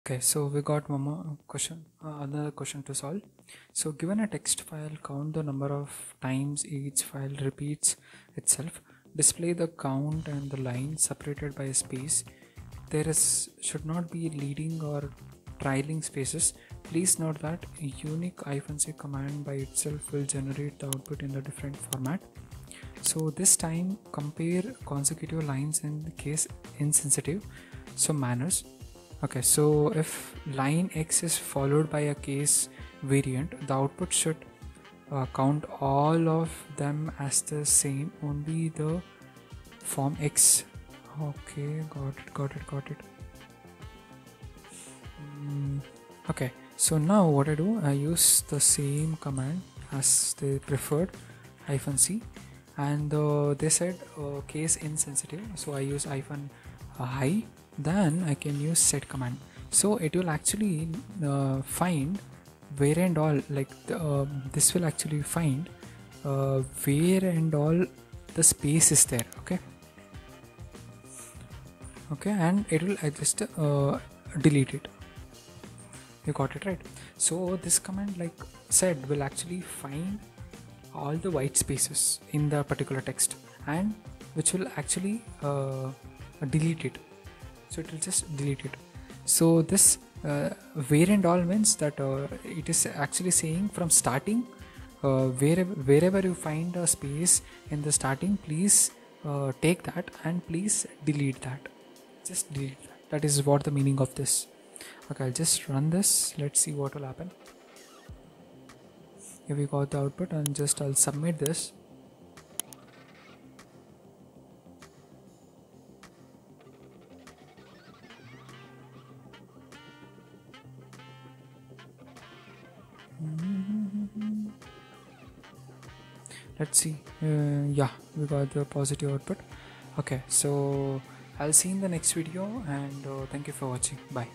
'Uniq' Command #3 | Hacker Rank Bash Problem solving
Master the art of shell scripting as we unravel and conquer a series of Hacker Rank challenges, equipping you with essential problem-solving skills for Linux and Unix-based systems.